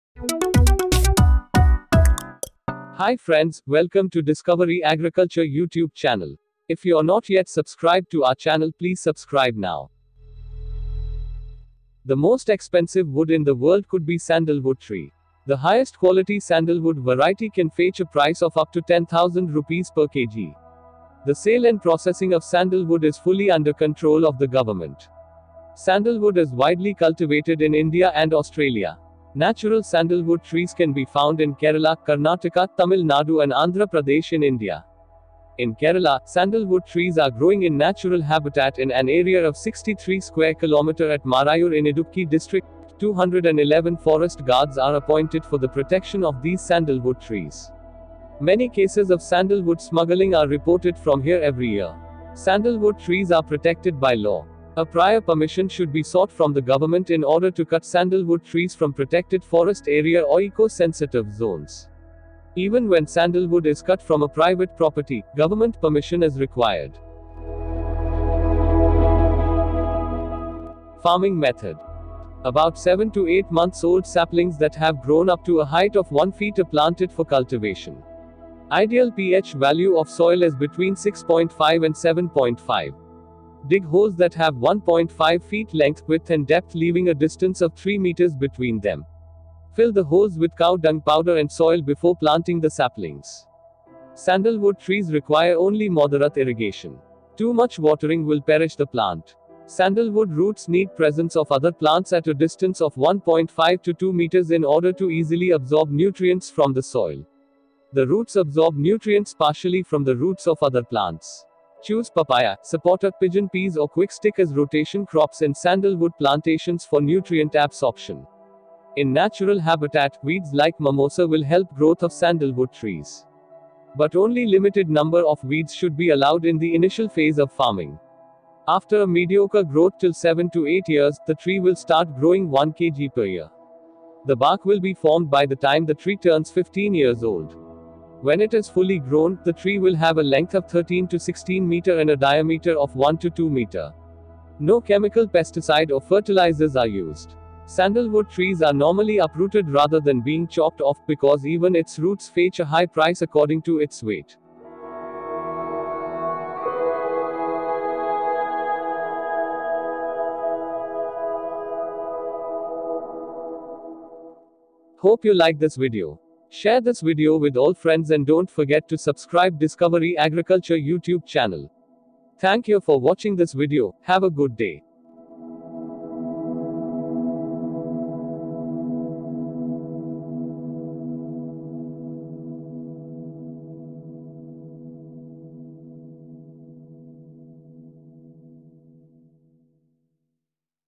SANDALWOOD FARMING / SANDALWOOD CULTIVATION
The most expensive wood in the world could be sandalwood trees. The highest quality sandalwood variety can fetch a price of up to Rs 10,000 per kg. The sale and processing of sandalwood are fully under the control of the government.

Sandalwood is widely cultivated in India and Australia. Natural sandalwood trees can be found in Kerala, Karnataka, Tamil Nadu and Andhra Pradesh in India.

In Kerala, sandalwood trees are growing in their natural habitat in an area of 63 square kilometres at Marayur in the Idukki district. 211 forest guards are appointed for the protection of these sandalwood trees. Many cases of sandalwood smuggling are reported here every year. Sandalwood trees are protected by law.

Prior permission should be sought from the government in order to cut sandalwood trees from protected forest areas or eco-sensitive zones. Even when sandalwood is cut from private property, government permission is required.

Farming method

About 7-8 months old saplings that have grown up to a height of one foot are planted for cultivation. The ideal pH value of soil is between 6.5 and 7.5.

Dig holes that have 1.5 feet in length, width and depth leaving a distance of 3 metres between them.

Fill the holes with cow dung powder and soil before planting the saplings.

Sandalwood trees require only moderate irrigation. Too much watering will perish the plant.

Sandalwood roots need the presence of other plants at a distance of 1.5 to 2 metres in order to easily absorb nutrients from the soil.

The roots absorb nutrients partially from the roots of other plants.

Choose papaya, sapota, pigeon peas or quick stick as rotation crops in sandalwood plantations for nutrient absorption.

In the natural habitats, weeds like mimosa will help the growth of sandalwood trees. But the only a limited number of weeds should be allowed in the initial phase of farming.

After a mediocre growth of 7-8 years, the tree will start growing one kg per year.

The bark will be formed by the time the tree turns 15 years old.

When it is fully grown, the tree will have a length of 13-16 metres and a diameter of 1-2 metres.

No chemical pesticides or fertilisers are used.

Sandalwood trees are normally uprooted rather than chopped off because even their roots fetch a high price according to their weight.